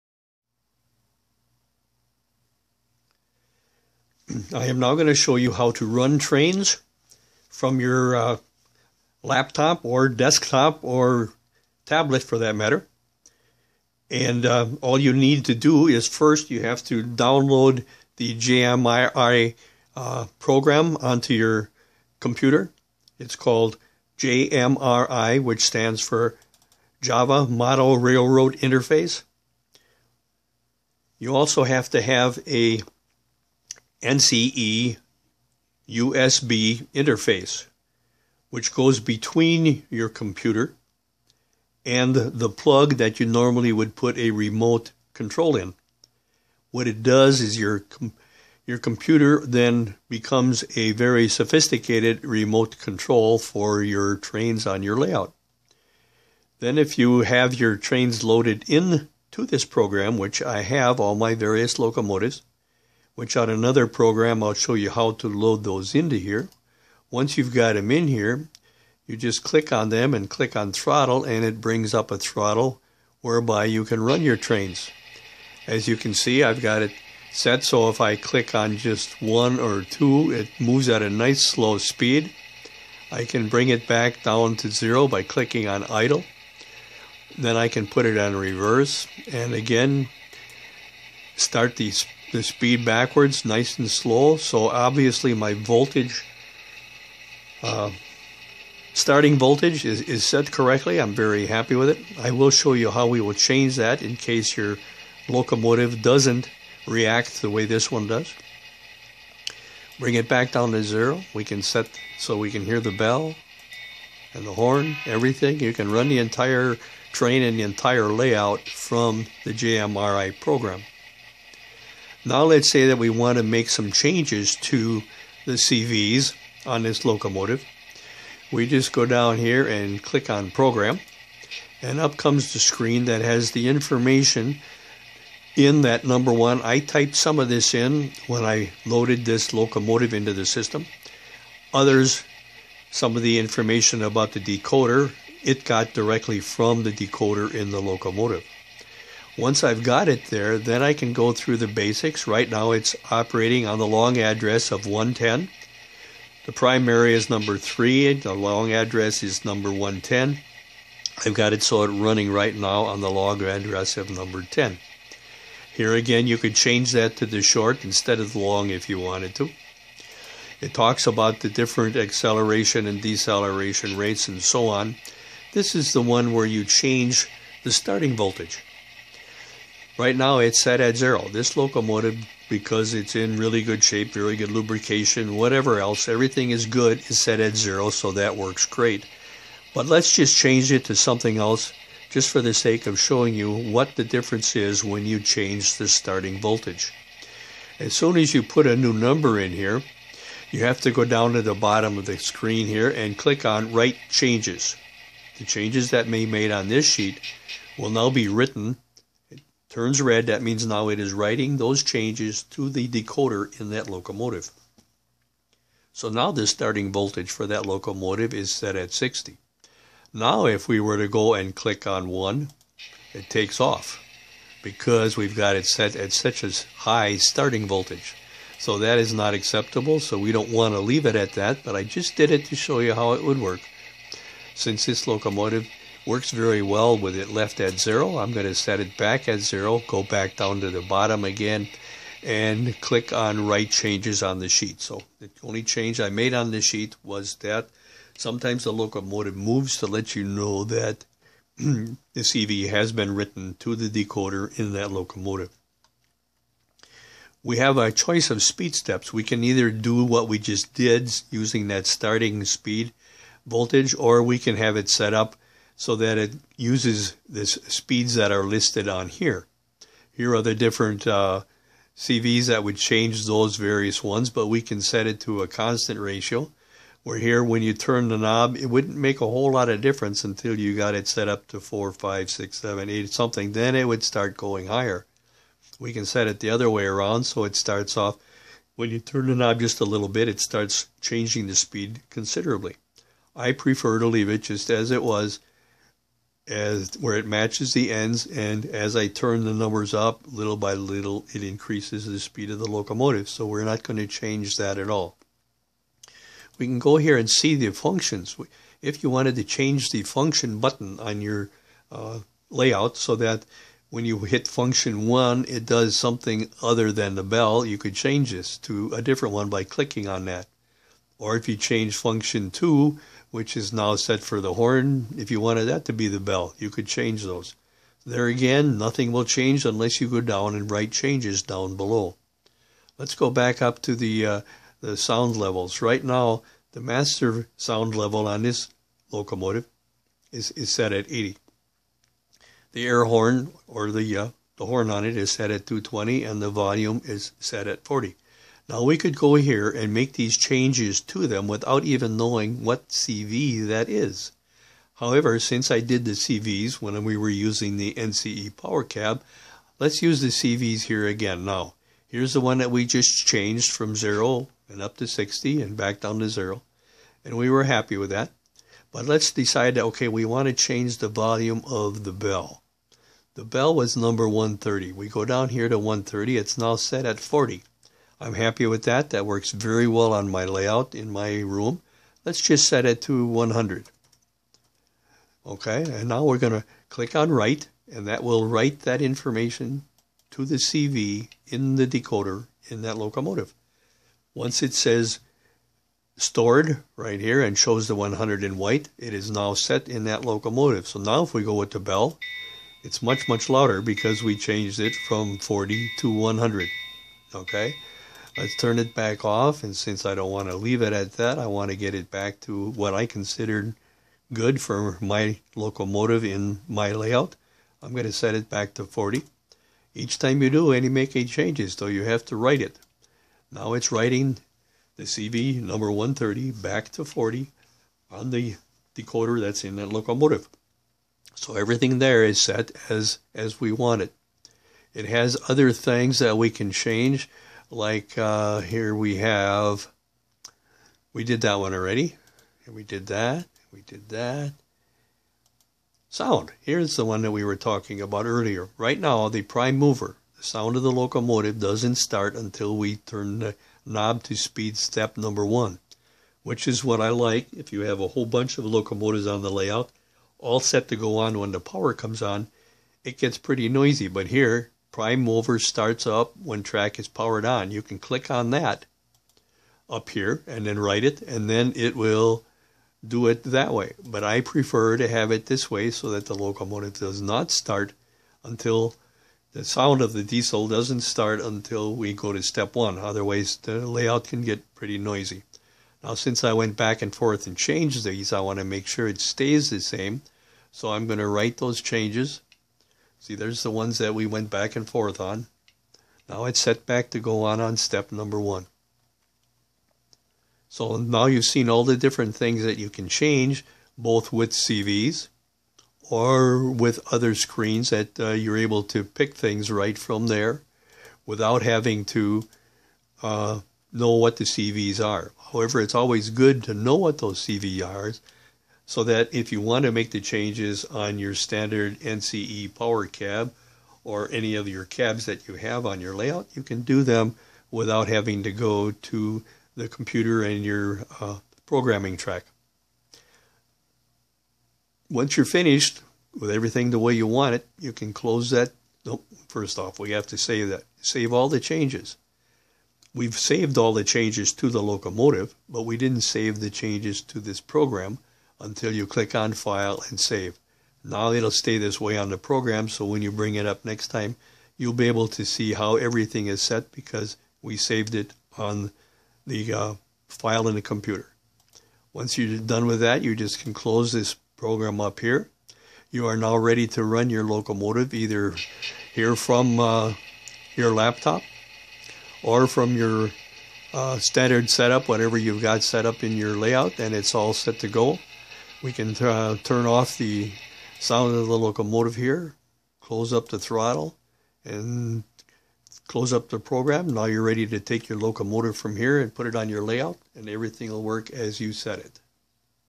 JMRI Decoder Pro to Change CVs
This Video shows how I change Decoder CVs using JMRI Decoder Pro and run trains from the same.